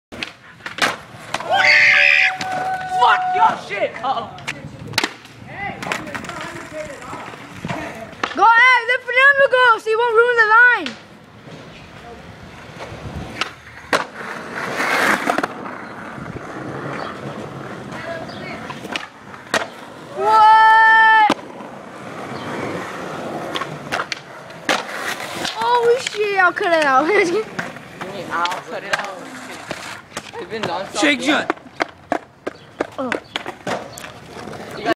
Skating Off the Four at Chaffey High School!
Skating at Chaffey High School
Robert Cabrera-HeEl Flip
Gustopo-Switch Ollie
Fernando Vega-Kick Flip
Andrew-Kick Flip Fail
Matthew Esparza-Ollie, SHAKE JUNT!